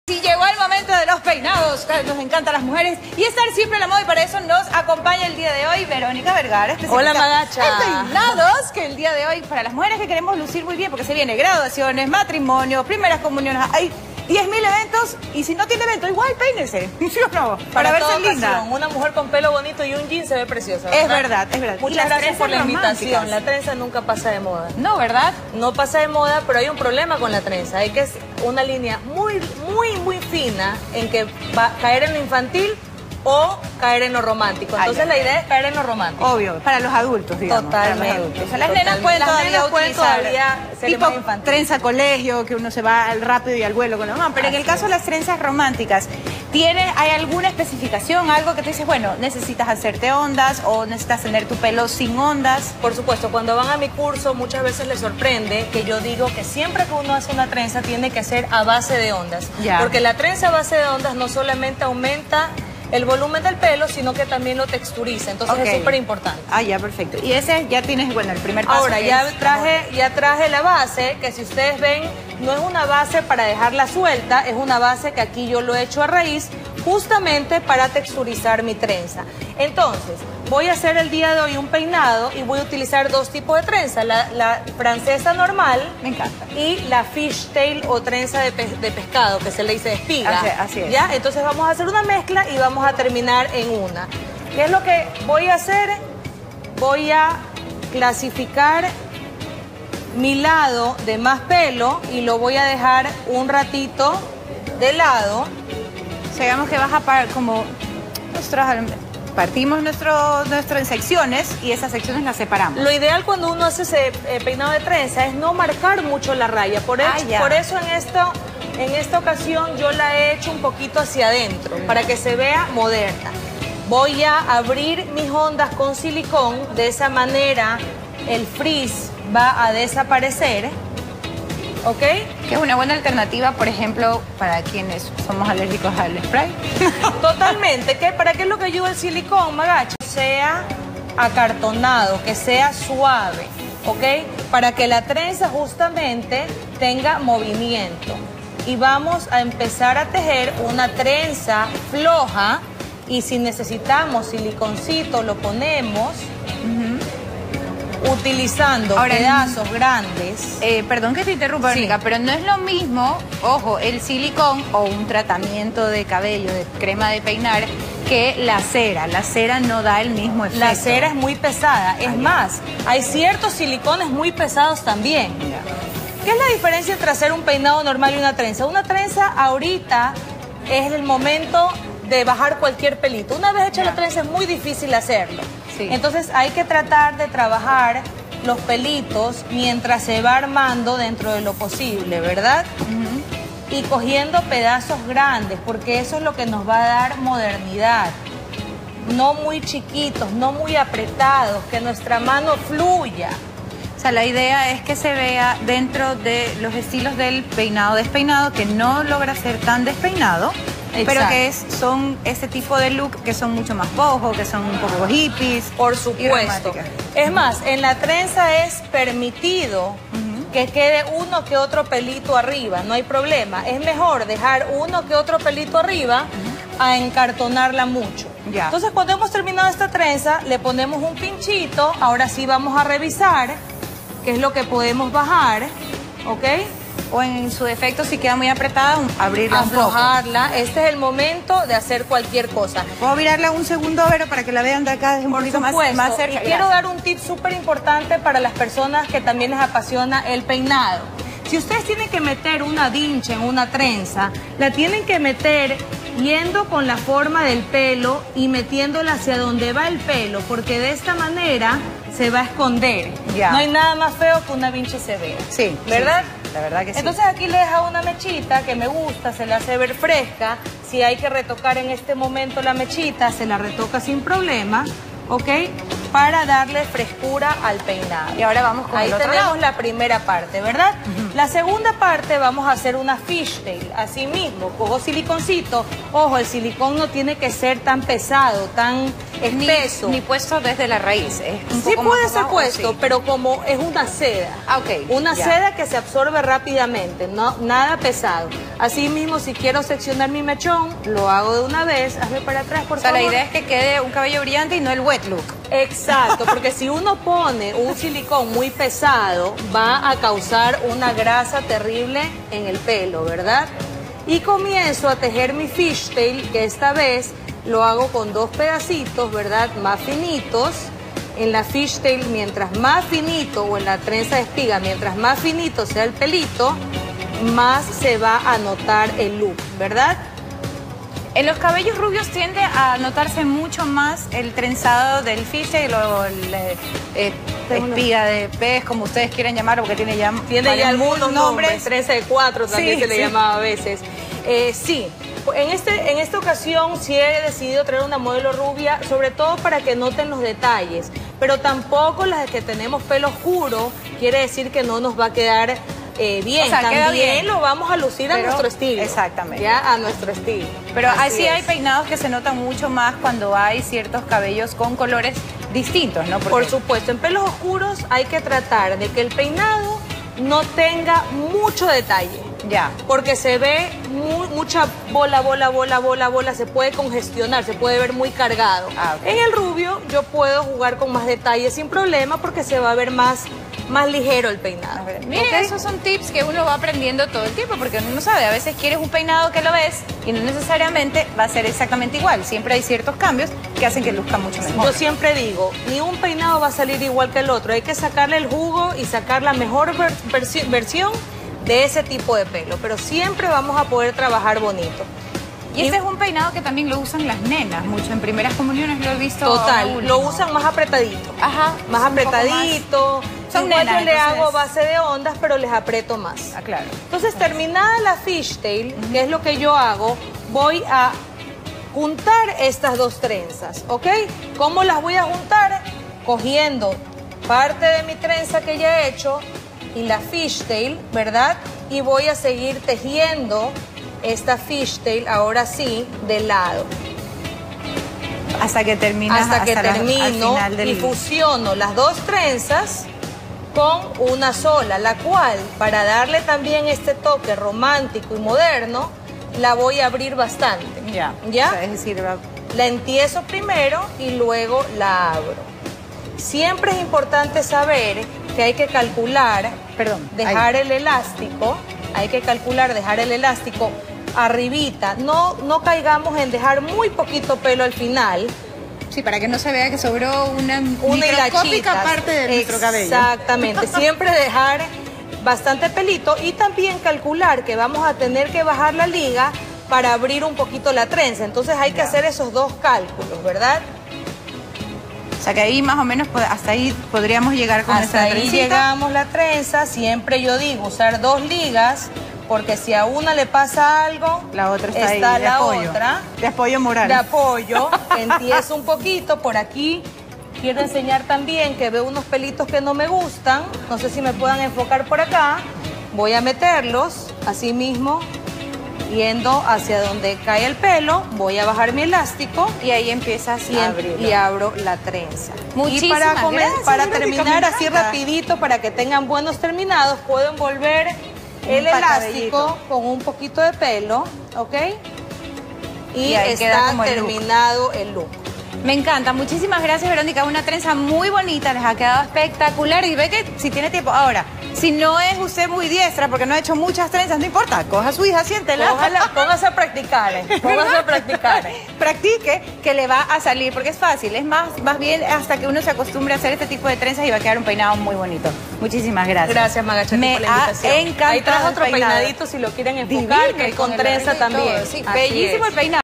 Hablamos sobre los 'peinados románticos con trenzas'
Para ello contamos con los consejos de la estilista, Verónica Vergara

Ese tipo de peinados son ideales para obtener una apariencia muy hermosa a la hora de asistir a cualquier evento social.